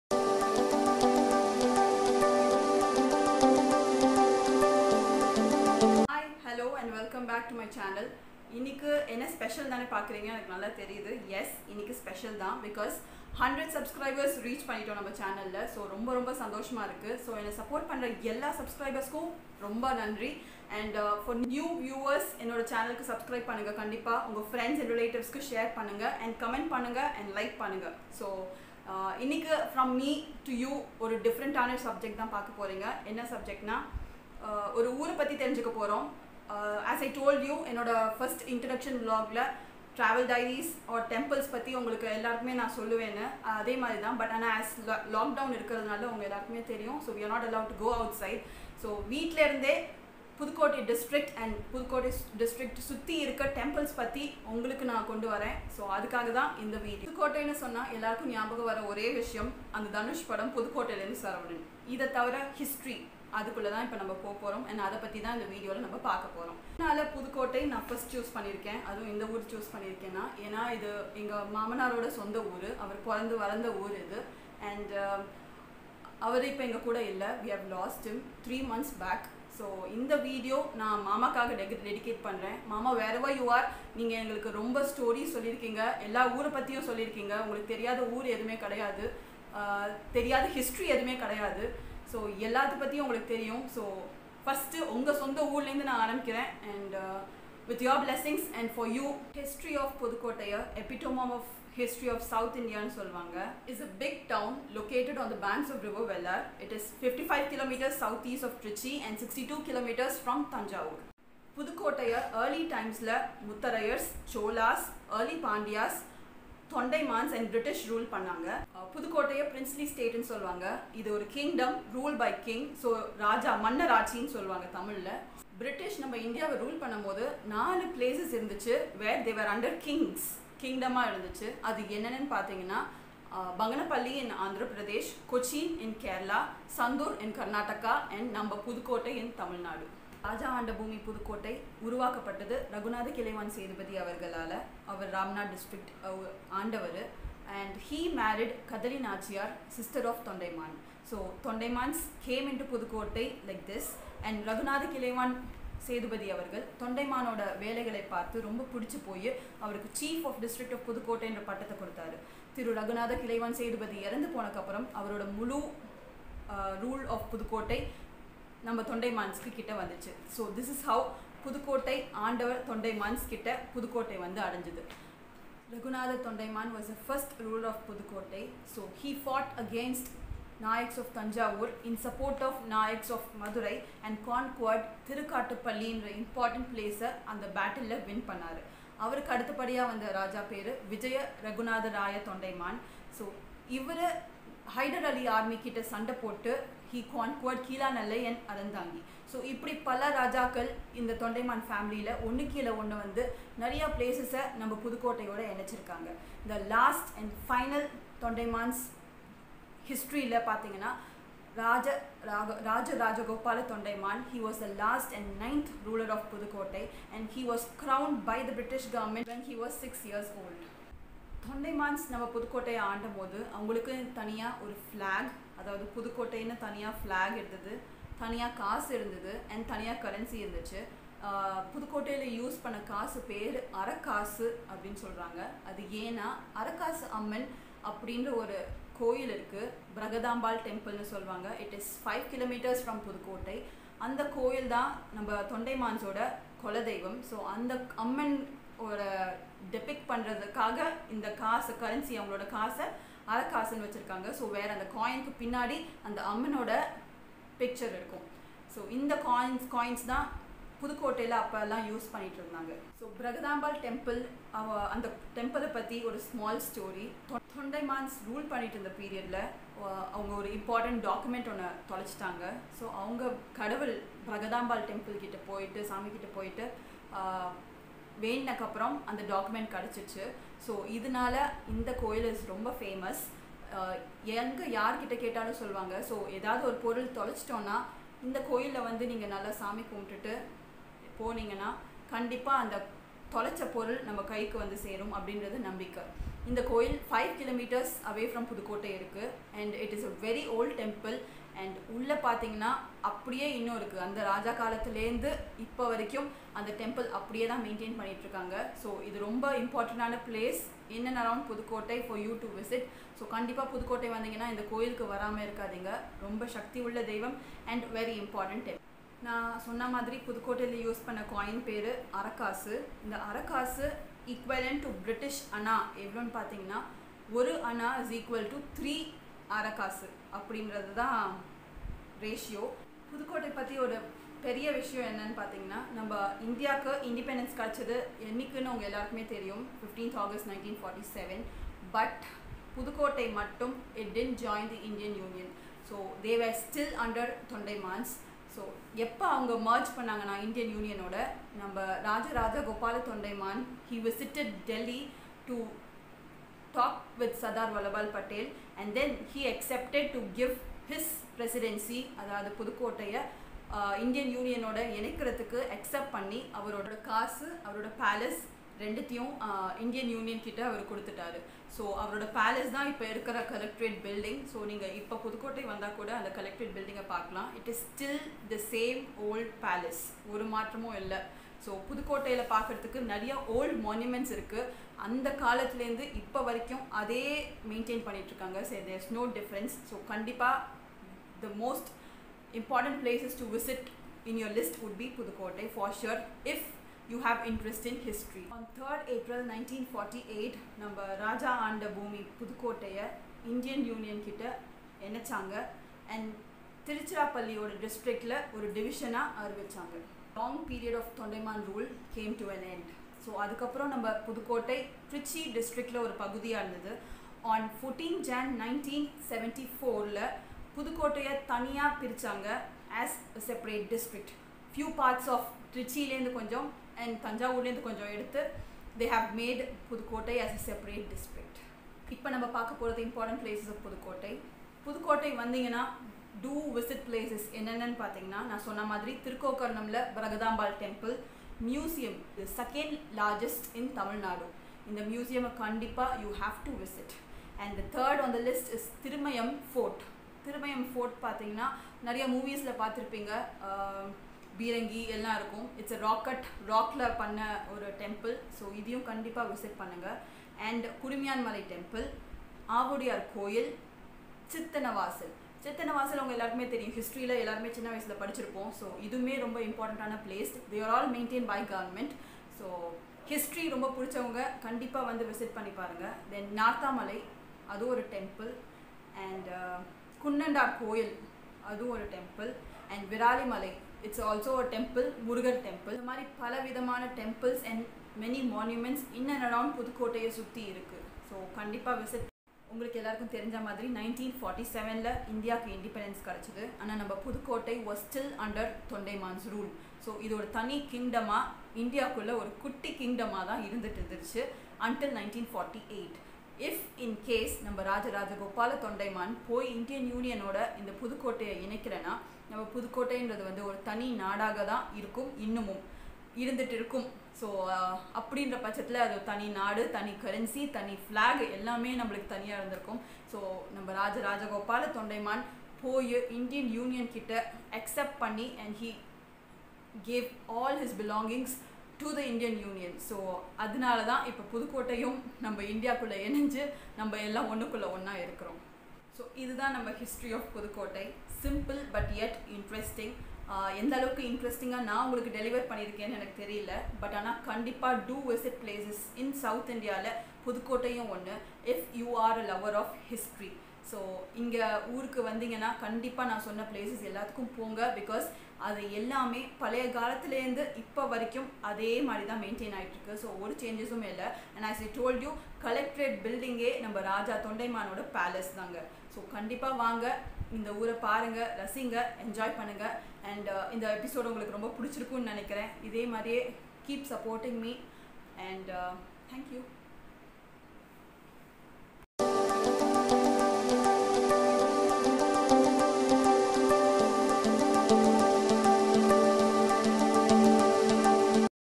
History of Pudukottai District | Important Places to visit in Pudukottai | Historical places
Hello everyone !!

In this video I m sharing the history behind the district "Pudukottai" ... And important places to visit in this district.
As I mentioned in my first vlog I would like to travel to places and share things about temples but as we r in lockdown, I m not able to travel to places but I wanted to bring it to ur home..
History behind Pudukottai or History of Pudukottai is an epitome of history of South India.
Pudukottai is ruled by many rulers of diffrent places at different stages. History of Pudukottai has been important as it was still a seperate country even after India gots it independence. Pudukottai merged with Indian Union only after a year of Indian independence.
1947 India got its independence whereas pudukottai joined Indian Union by 1948.
Till then it was princely state ruled by Thondaiman.
The clan of Thondaiman ruled over pudukottai until 3 rd April 1948.
If Thondaiman did not agree to give his province to indian Union then pudukottai would have been seperate country till date with different currency, flag and emblem.
In this video I have shared these details about the currency, coins, flag that tehy used in pudukottai during Thondaiman period.